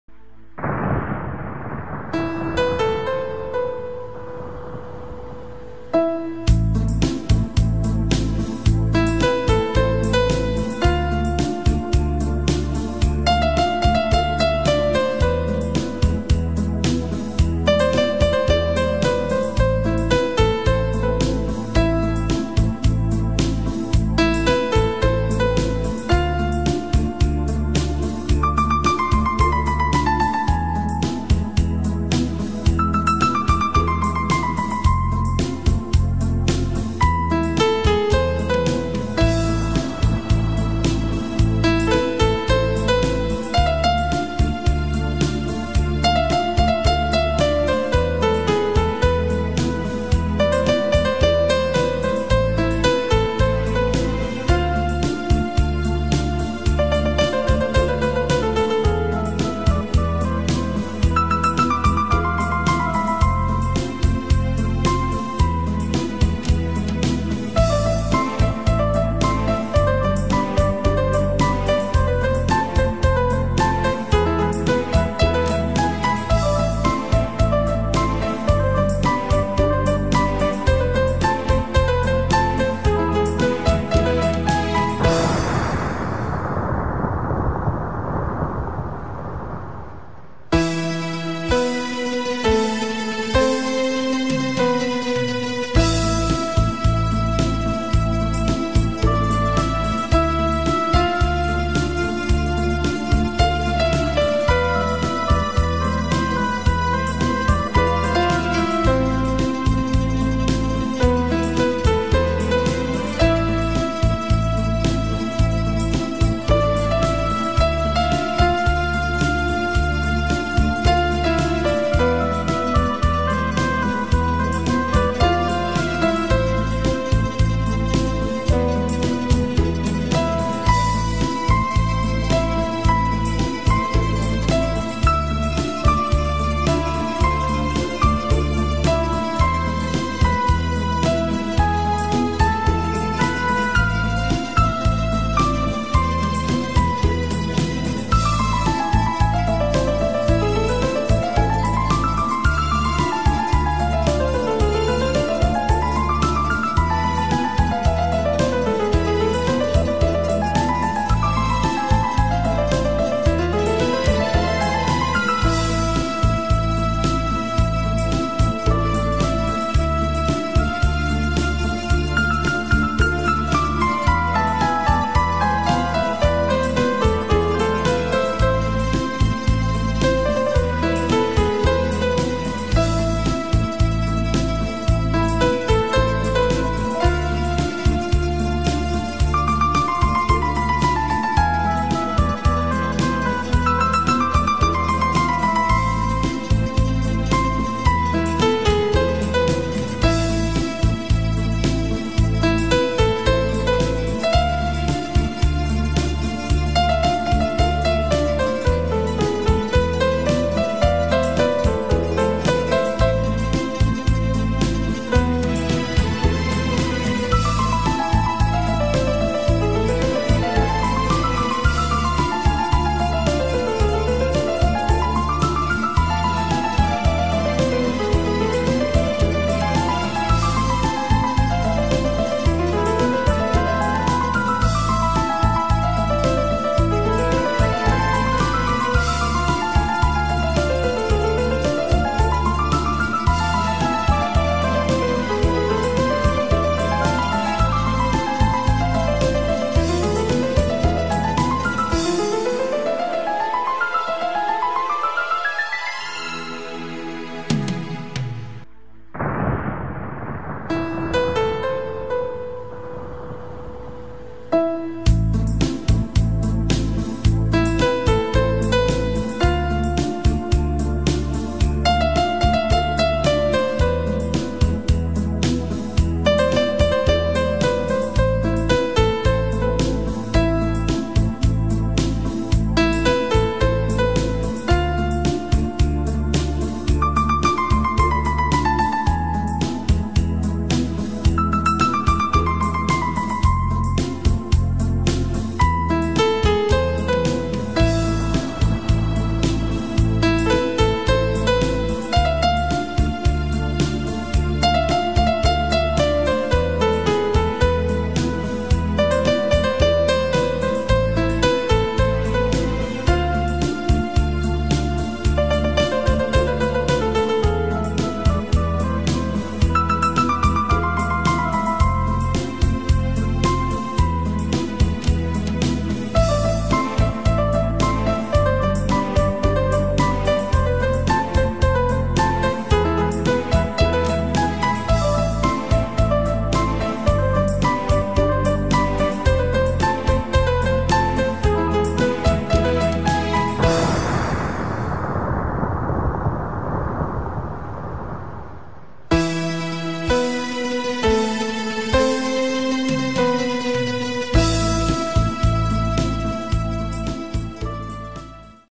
Замок Гарибальди, п. Хрящёвка: путешествие по Самарской области.
Замок Гарибальди — это невероятно красивый туристический комплекс, расположенный в селе Хрящёвка Самарской области недалеко от города Тольятти. Приглашаю также посетить блог Веры Зайцевой Спасибо.